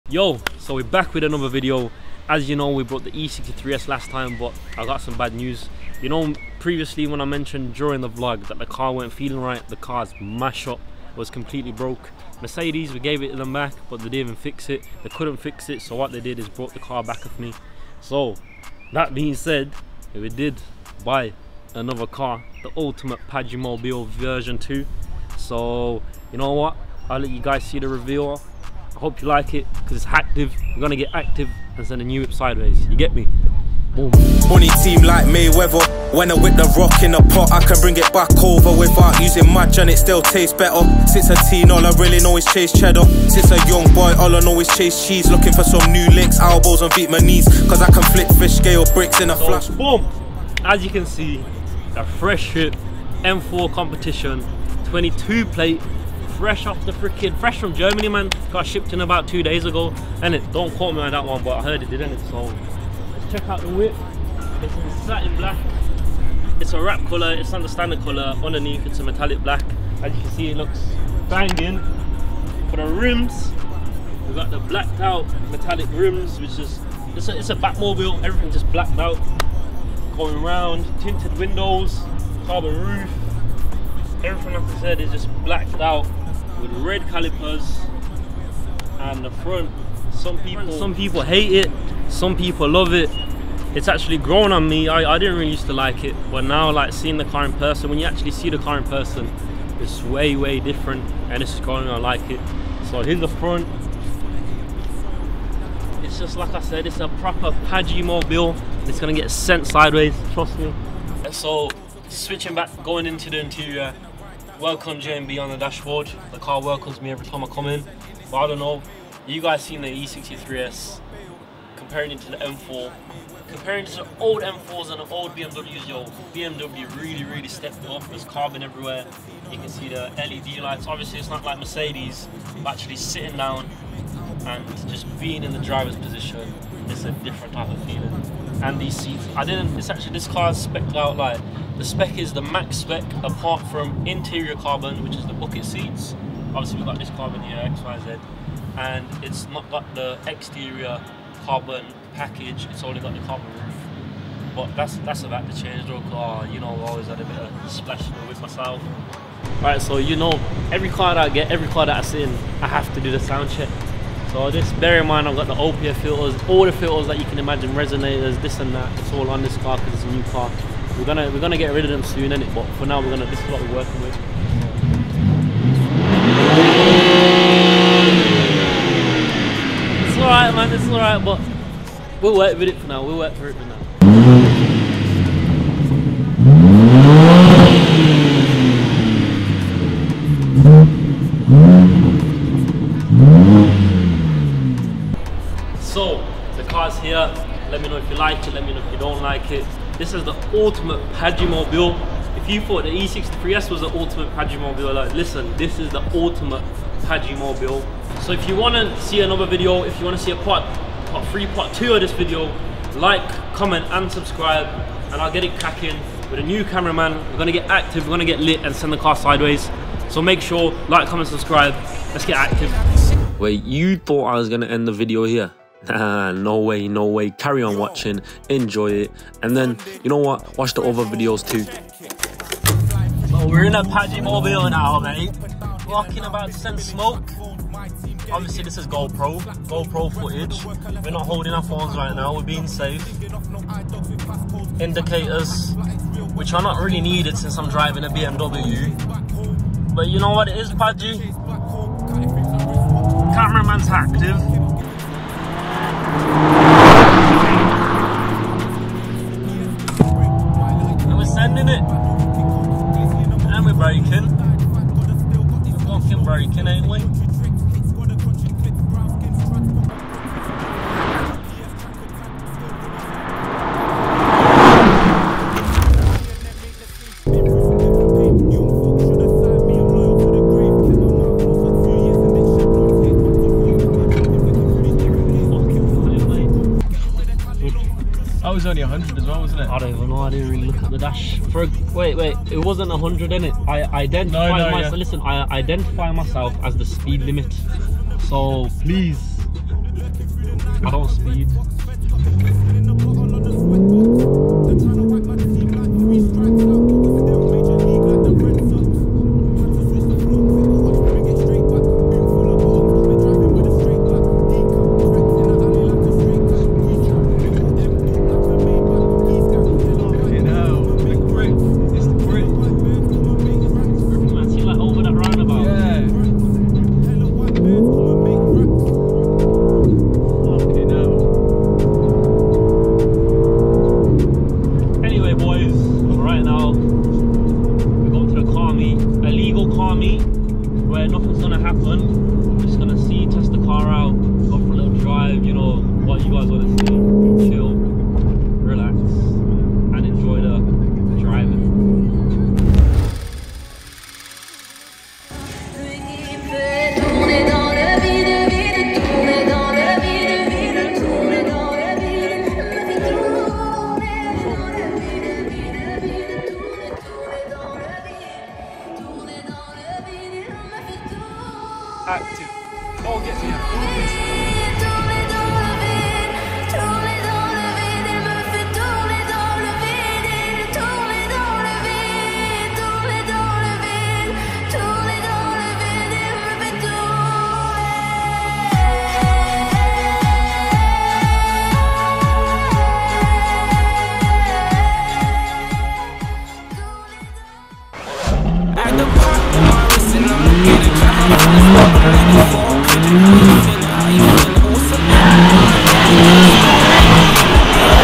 My E63S broke so I brought a G82 M4 Competition
My E63 broke so I bought a new G82 M4  competition.

Paji mode Hactivateddddd💨

+ Don't forget to add us on socials:
JayemB-

& CHOP LIFE 💎